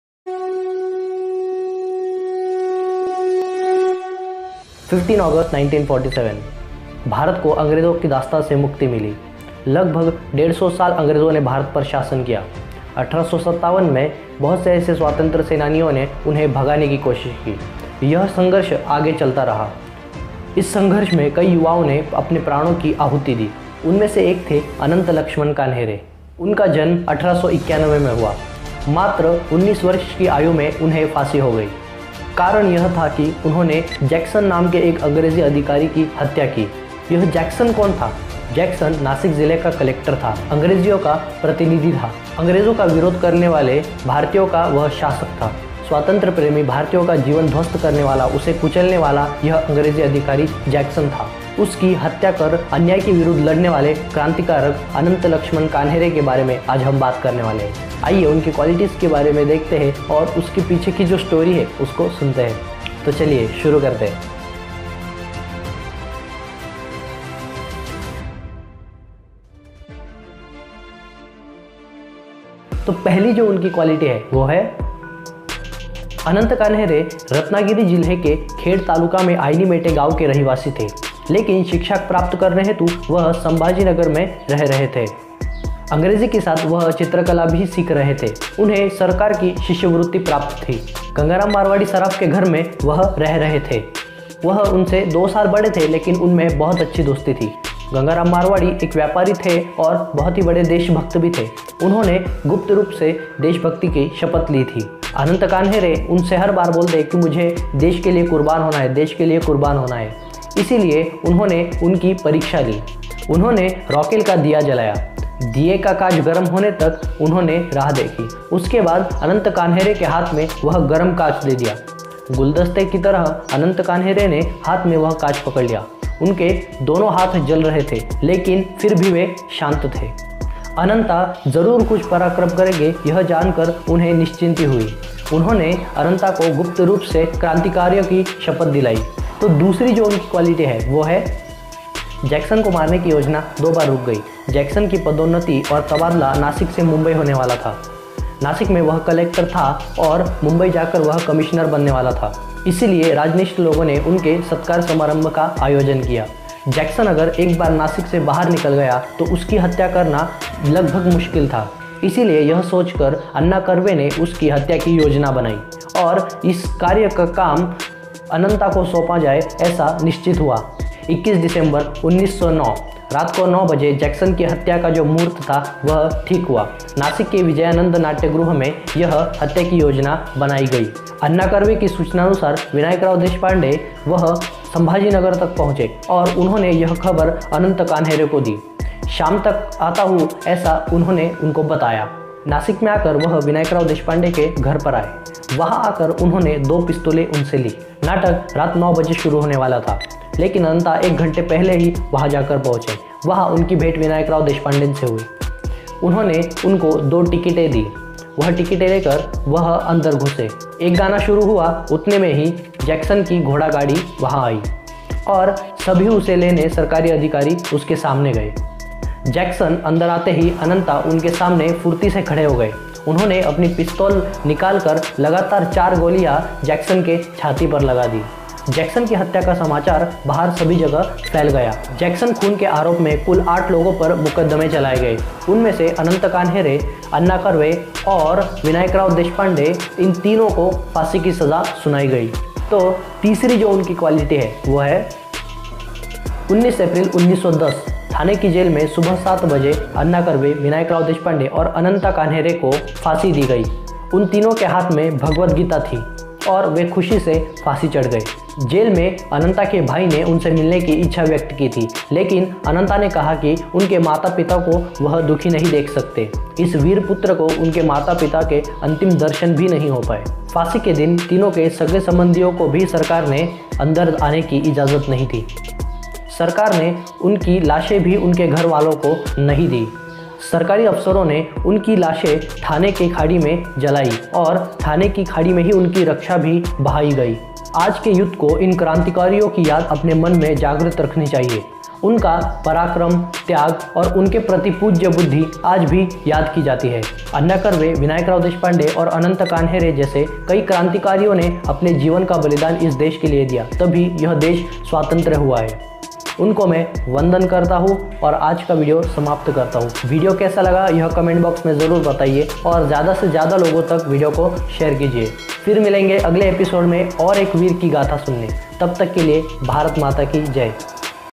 Ep#21|Youth Icon|Anant Laxman Kanhere|Freedom fighter
About this Video: Lessons From Ananta kanhere
     Anant Laxman Kanhere ( हुतात्मा अनंत लक्ष्मण कान्हेरे) was an Indian independence fighter. On 21 December 1909, he shot the Collector of Nashik in British India. The murder of Jackson was an important event in the history of Nashik and the Indian revolutionary movement in Maharashtra.